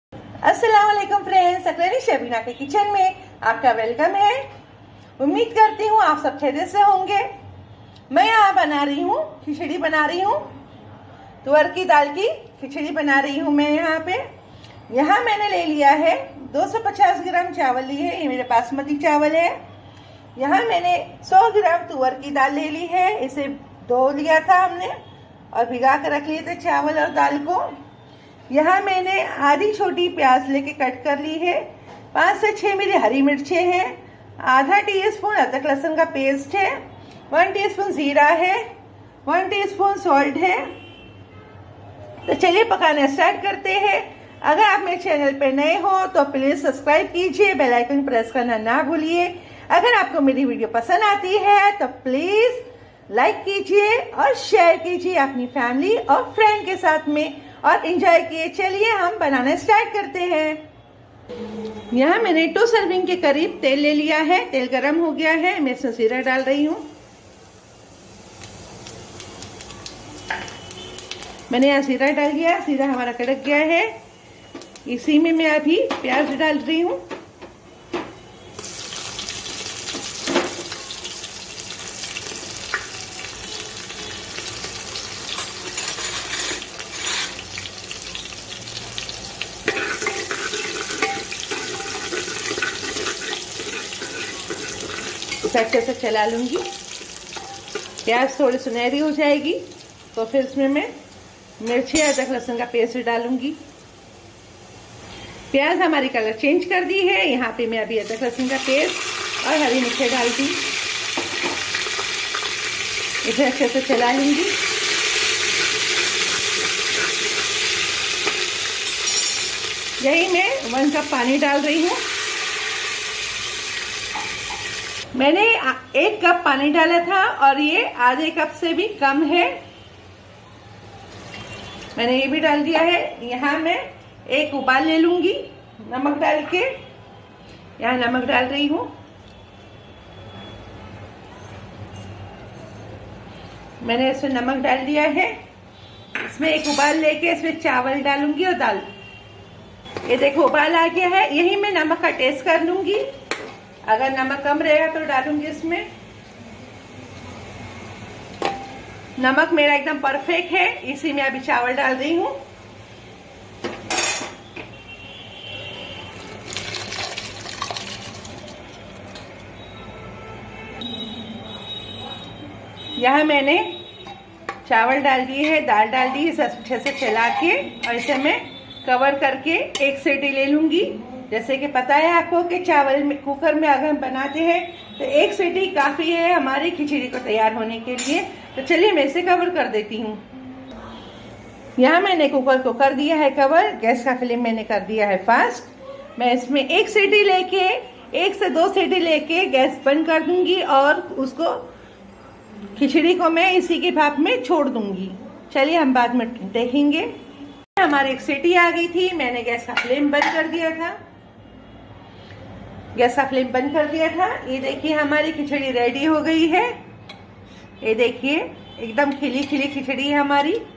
Arhar Dal Ki Khichdi ( Different & Easy Recipe) BY SSNQ
ARHAR DAL KI KHICHDI

INGREDIENTS:-
250 GM RICE
100 GM ARHAR KI DAL
5 HARI MIRCHI
1 TSP ZEERA
1/2 Medium onion
1 tsp salt
1/2 Tsp ginger garlic paste
2 serving sp Oil